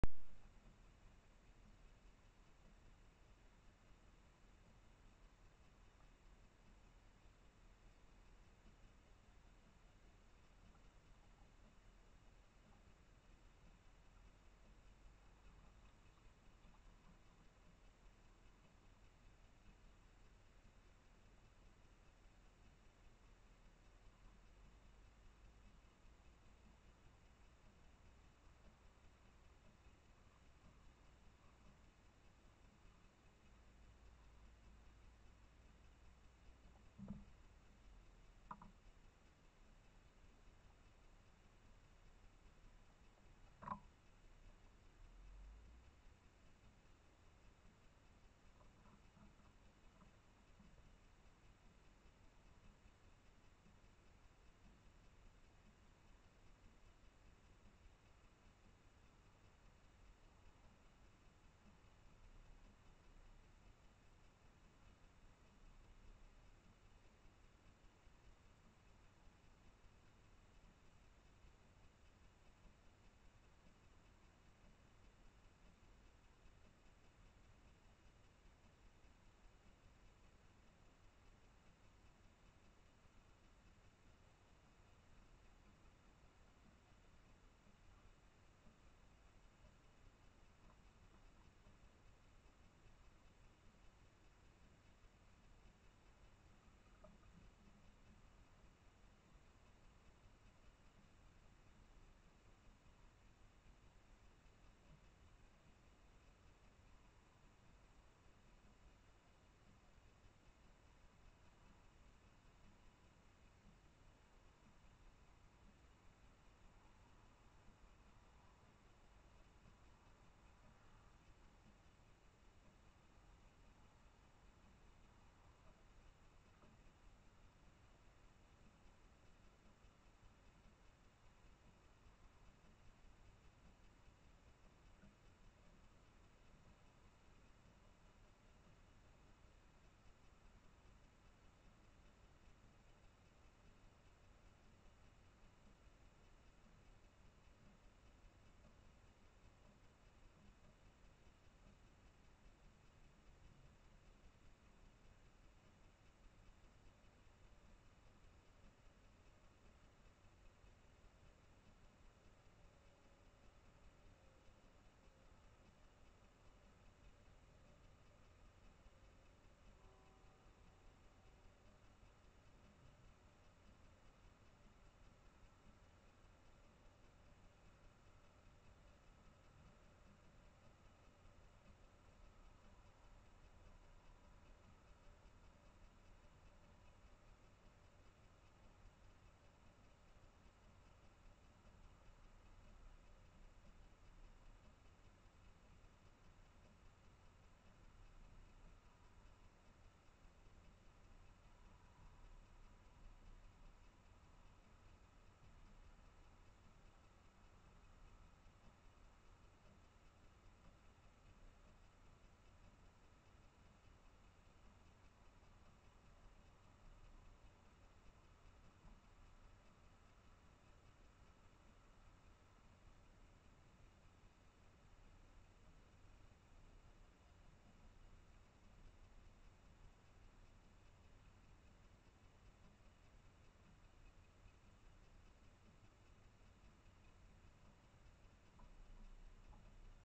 ASMR - Unser Siebenschläfer 04 3
Aufgenommen mit der Wild Kamera in unserem großen Garten in den Weinbergen.